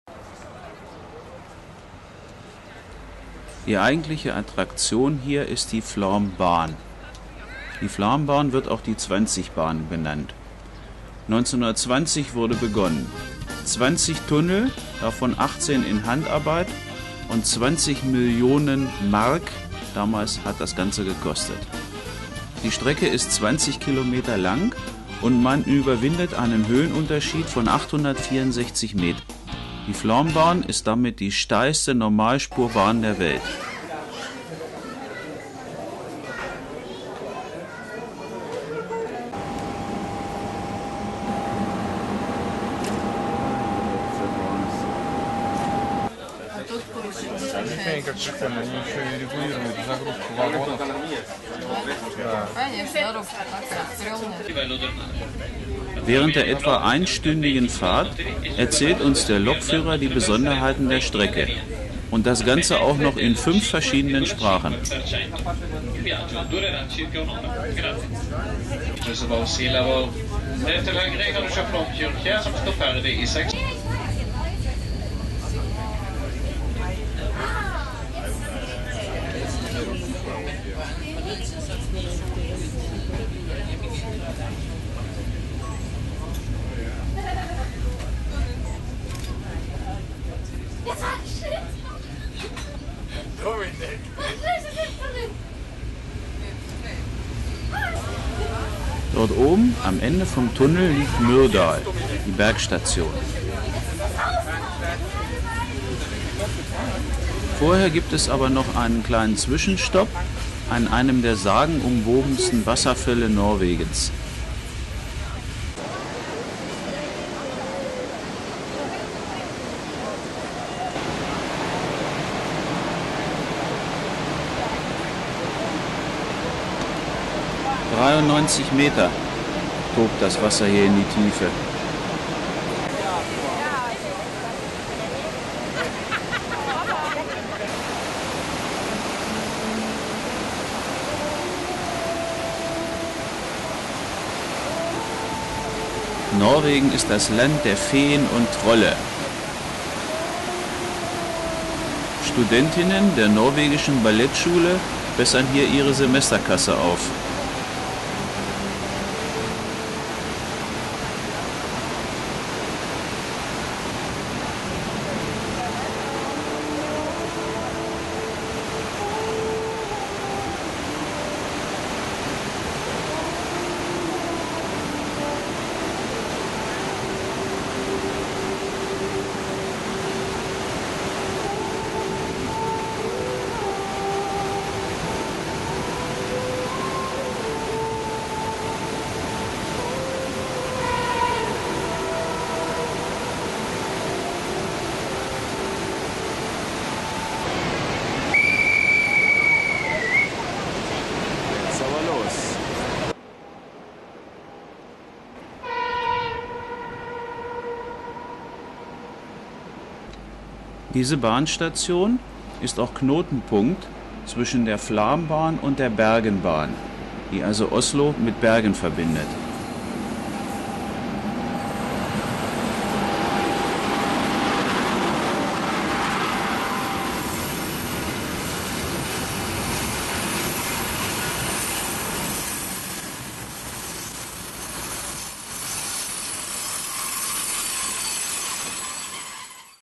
Flam Bahn
Die Flåmsbahn (norw. Flåmsbana) ist eine eingleisige normalspurige Stichstrecke der norwegischen Bergenbahn. Sie führt vom 866 m hoch gelegenen Myrdal hinunter durch das Tal Flåmsdalen nach Flåm am Aurlandsfjord.
Die Bahnstrecke ist eine wichtige Touristenattraktion Westnorwegens und wird dementsprechend auch hauptsächlich von Touristen benutzt. Im Jahr 2005 gab es 475.000 Reisende. Beim Wasserfall Kjosfossen wird ein Stopp eingelegt, um den Touristen ausreichend Möglichkeit zum Fotografieren zu geben.